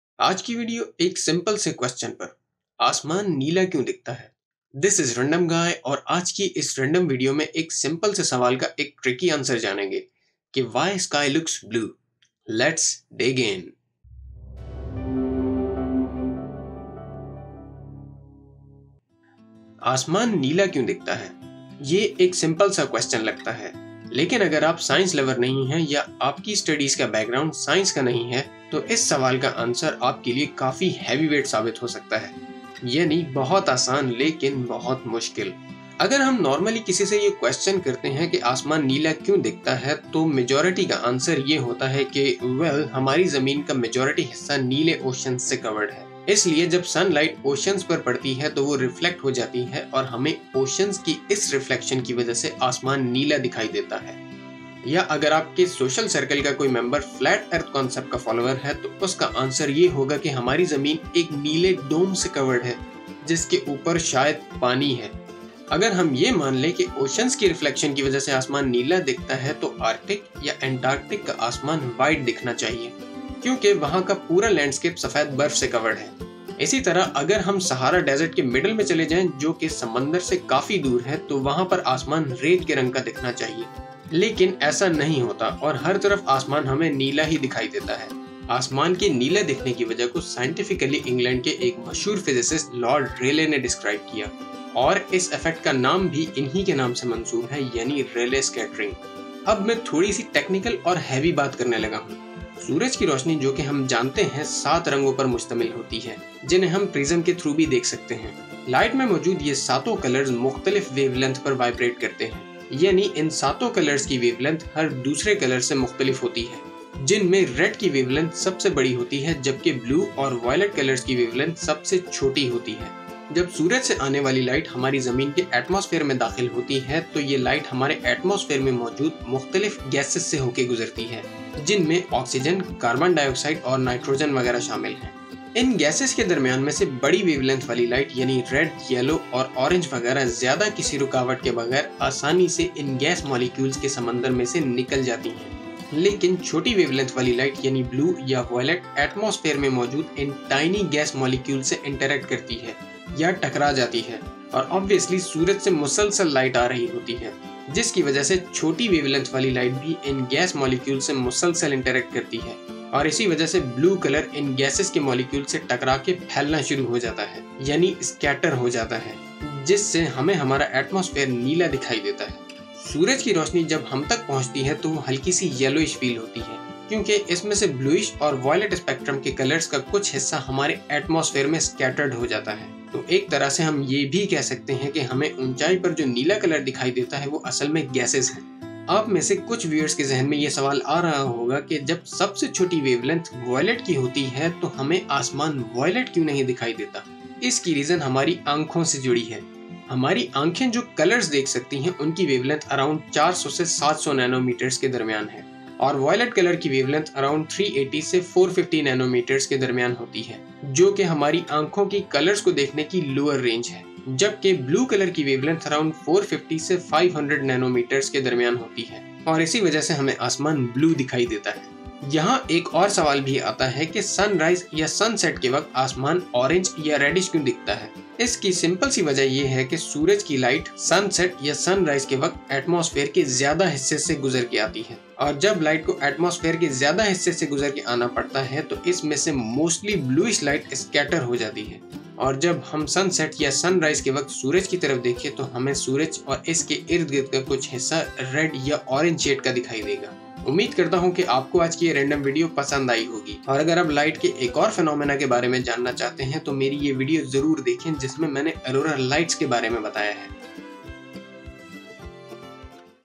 Why Sky looks Blue ??? (Urdu/Hindi)
In this video I have answered a very frequently asked question regarding sky's blue hue. Lots of people have misconception about blue hue of sky and the actual facts might be surprising and could be a bit heavy for you.